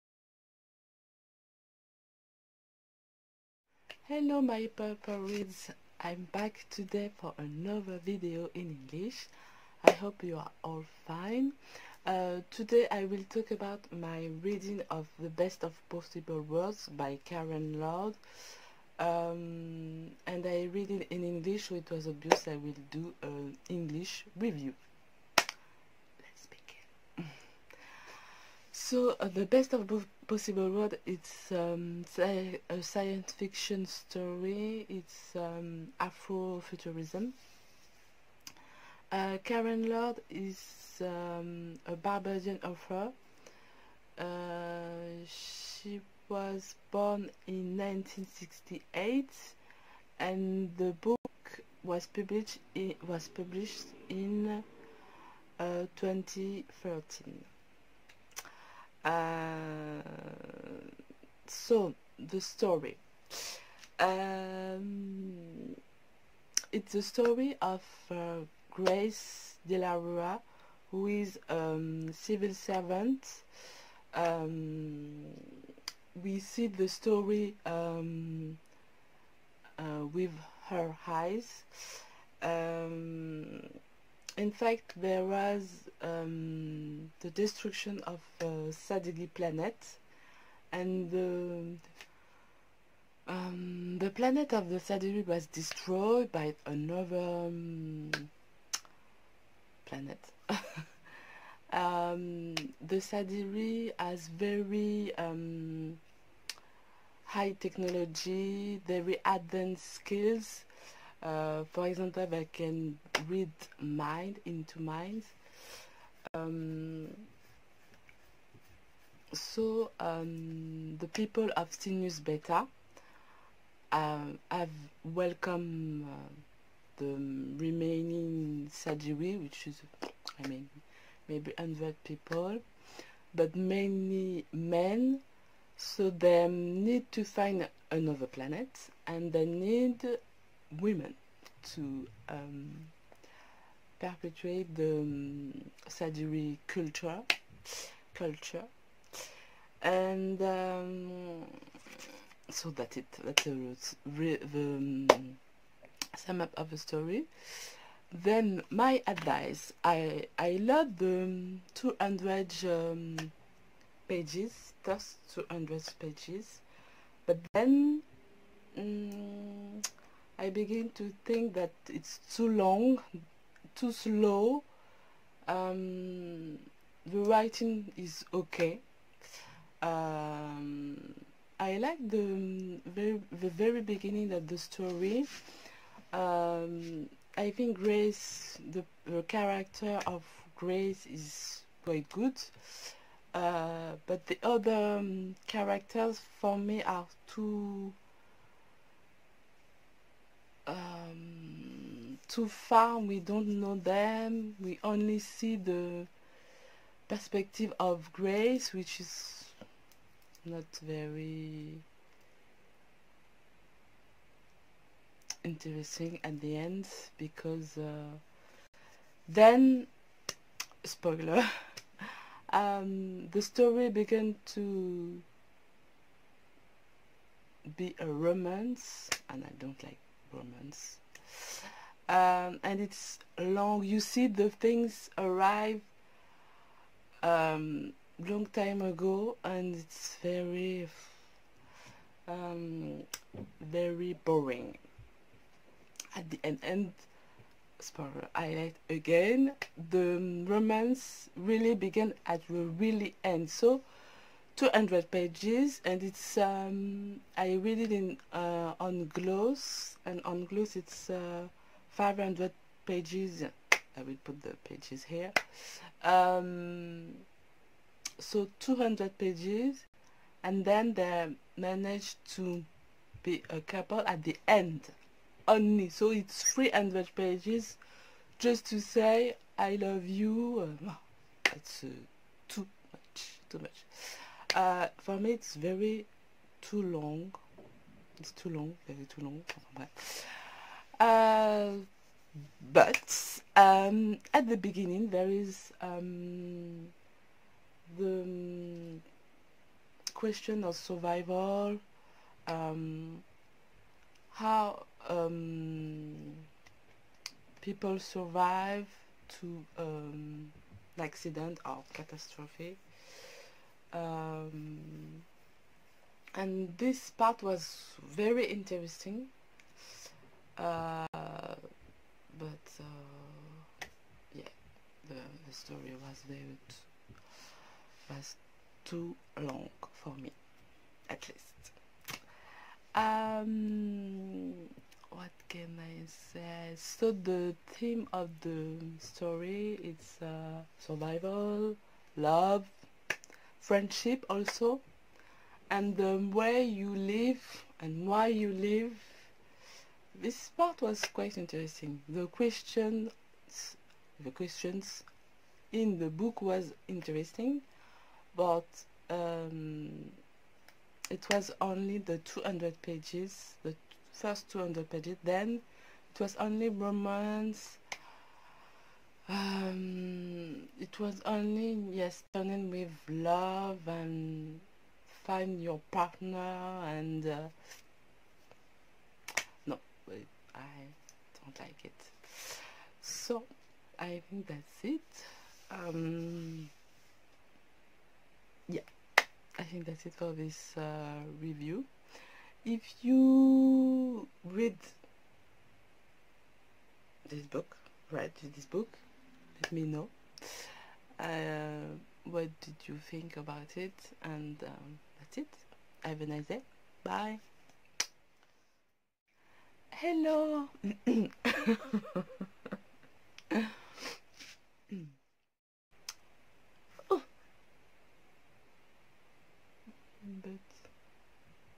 [ENG] AVIS/ The Best of all possible worlds - Karen Lord
Coucou mes roseaux,

Aujourd'hui, je viens avec une revue en anglais de The best of all possible worlds de Karen Lord que j'ai lu dans le cadre du caribathon mais avec du retard.

A bientôt